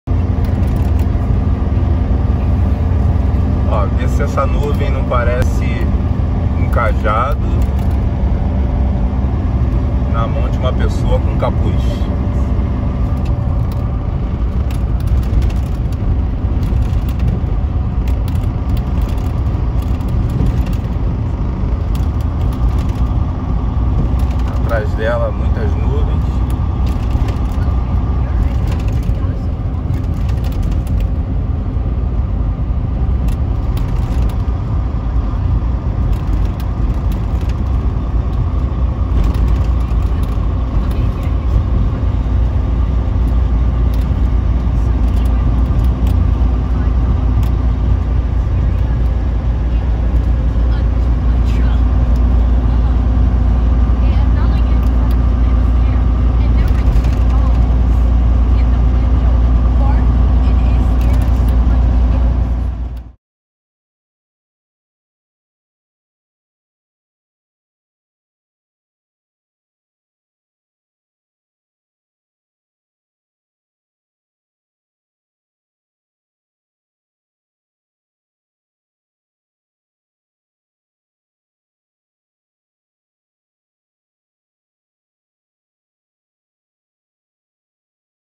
Ep 128. OBSERVE O FORMATO DA NUVEM NA FILMAGEM E NAS FOTOS NO CÉU DE OHIO🇺🇸I-80E
Sendo coincidência ou não, acredito que devemos orar não só pelos Estados Unidos, mas por todos os países do mundo.